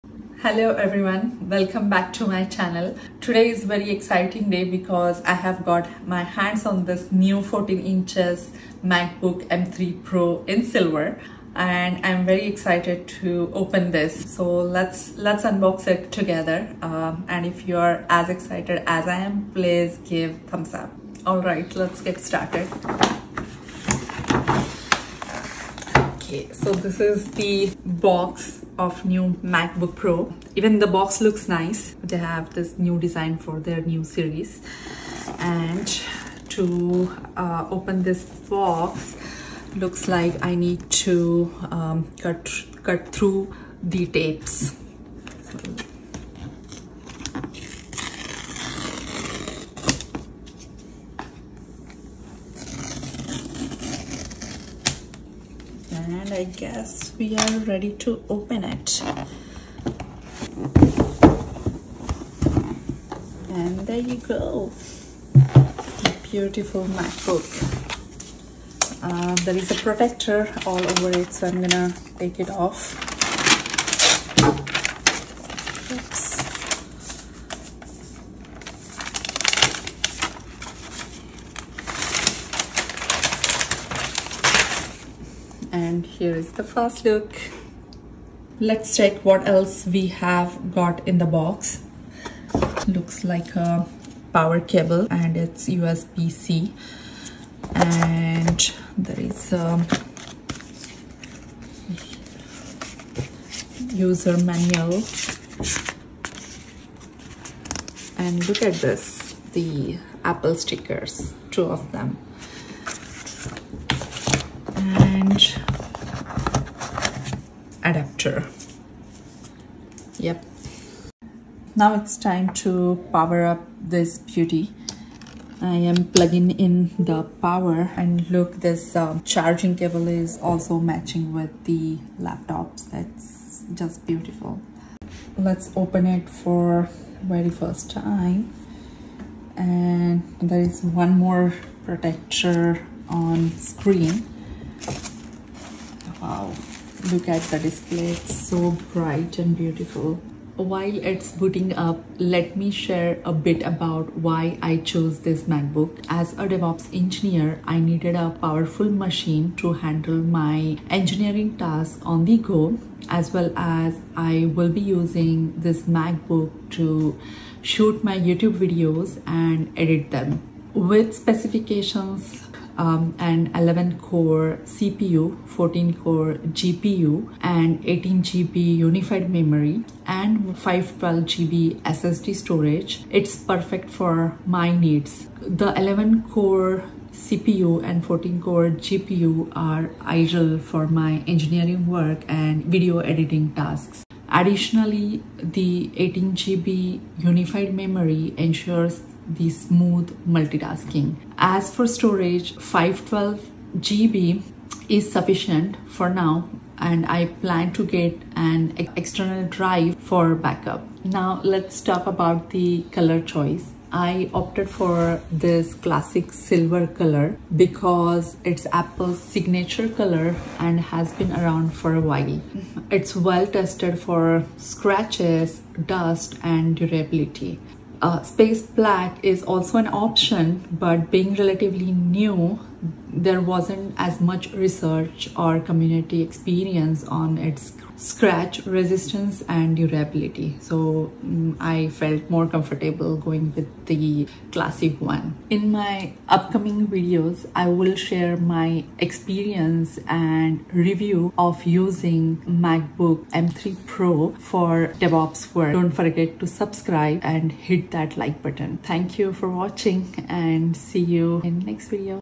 Unboxing the MacBook Pro M3 for Software Engineering Work!
Unboxing the MacBook Pro M3, the silver variant radiates elegance and complements Apple's iconic design. Tailored for software engineering, the M-series processor, ample RAM, and storage ensure seamless multitasking. Ideal for video editing with impressive graphics and a vibrant Retina display. The silver color enhances aesthetics and provides color accuracy. Optimized for content creation, the MacBook Pro M3 is a powerful and stylish tool for professionals in various creative fields.